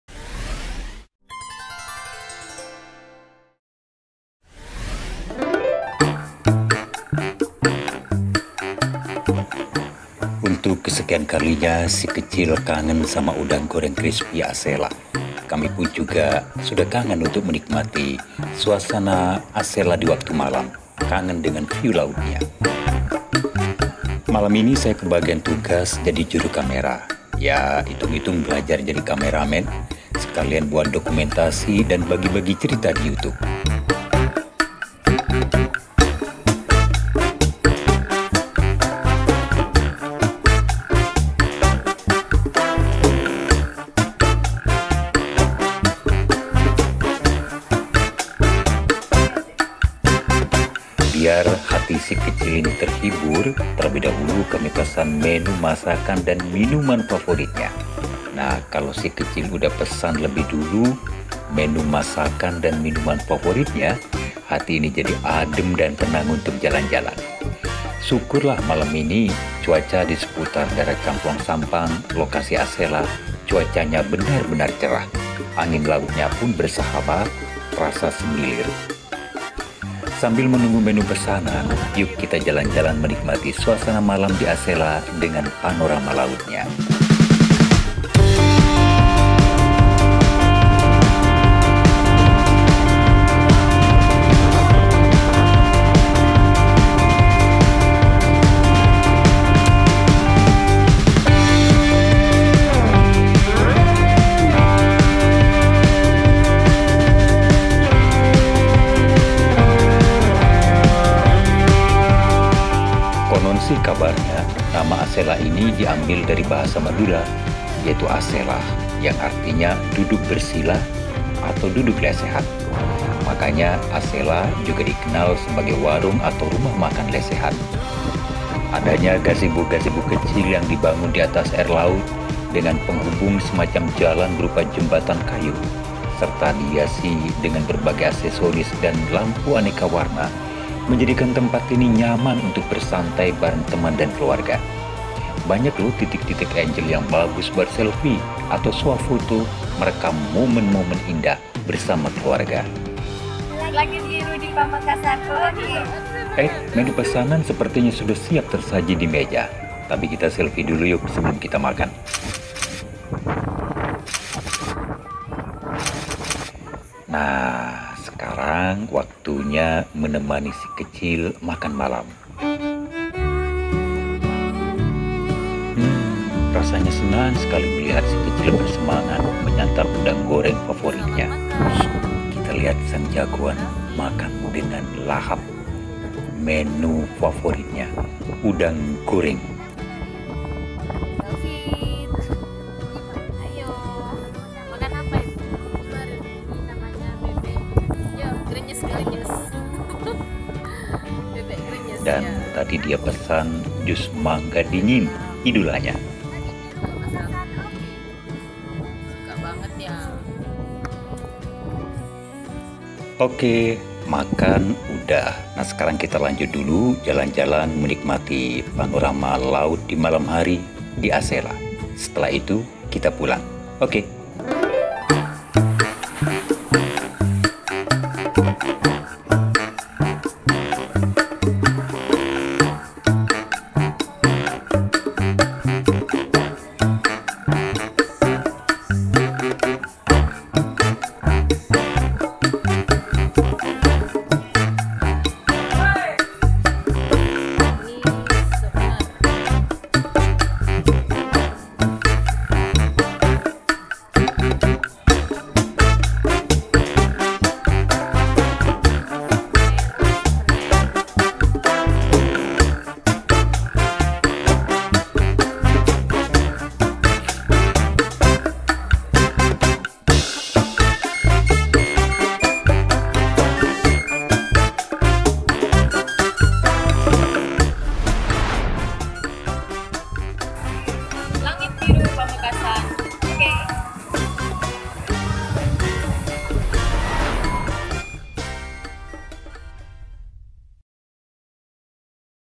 KANGEN ASELA - Rumah Makan TENGAH LAUT DIWAKTU MALAM - Sampang
ASELA Sampang.

Kami sekeluarga rasanya Kangen,ingin Menikmati Masakan Khasnya serta Suasana Malam dengan Panorama Laut nan Mempesona.
Rasa Rindu Pada  ASELA,Langit Biru Pamekasan wujudkan dalam Video KANGEN ASELA  warung kuliner di TENGAH LAUT.
Langit Biru TV menyajikan video tentang Perjalanan & acara, Vlog harian, Sejarah, Budaya, Wisata, Kuliner, Edukatif, Informatif.